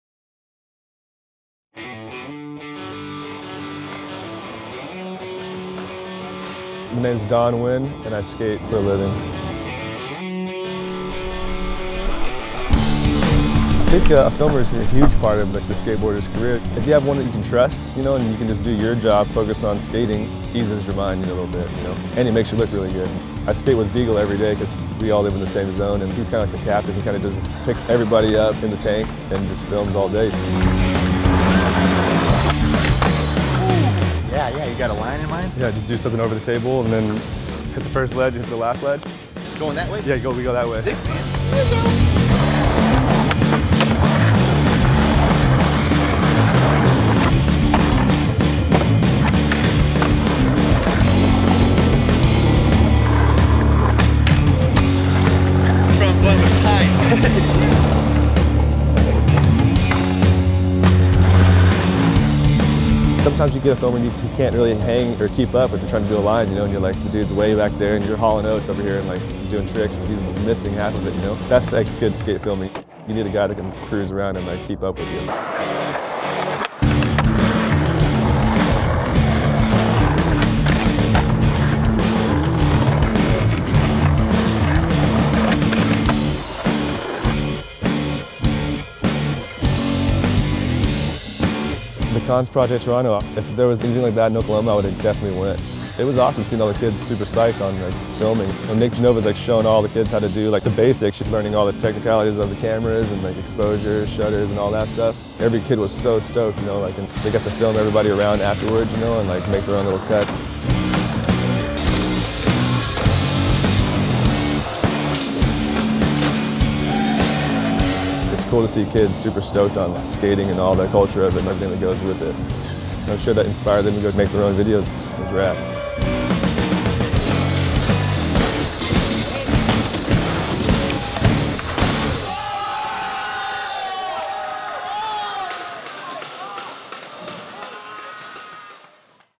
How to Film Skateboarding with Don "Nuge" Nguyen
This is a four part series for Cons Project, providing a DIY lesson on how to film a skate video. Four films will show you tips on skate filming including editing techniques, follow filming, shot composition, and skating in front of the camera with experts Mike Gilbert, Ewan Bowman, Nick Genova, and Don “Nuge” Nguyen.

Directed by Jon Holland